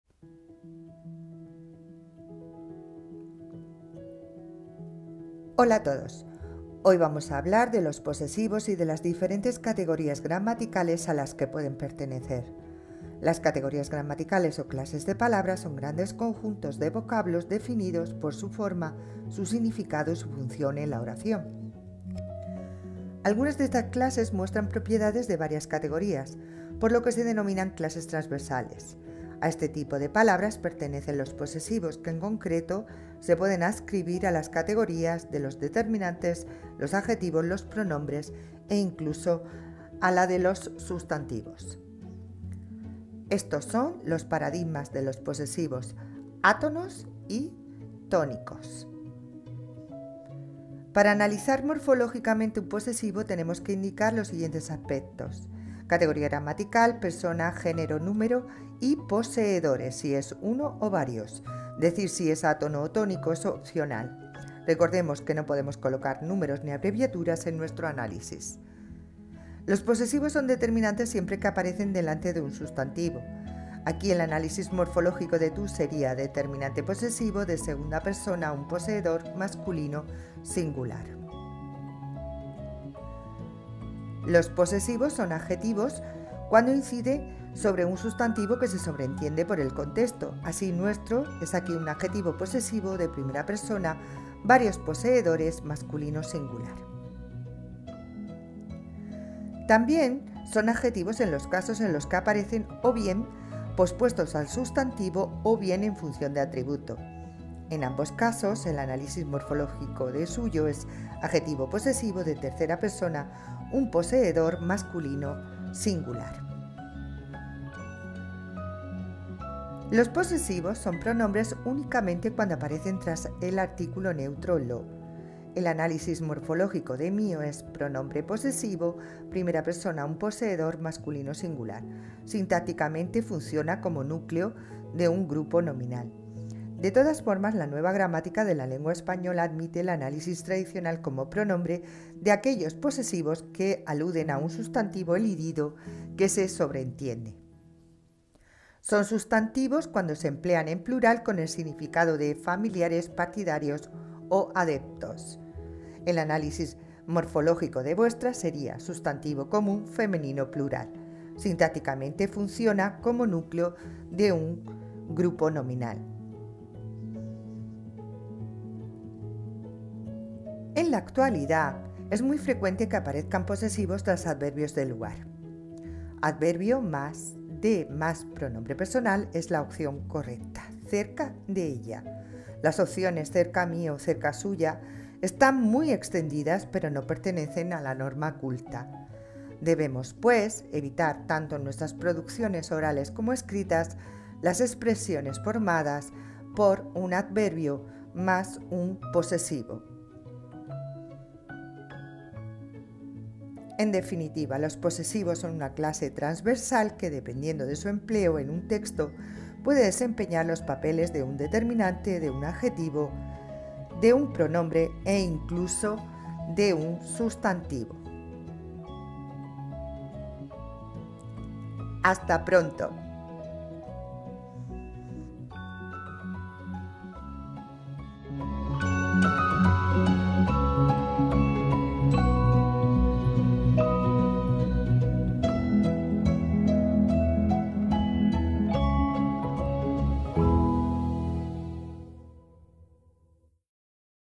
LOS POSESIVOS
Análisis morfológico y sintáctico actualizado de los POSESIVOS.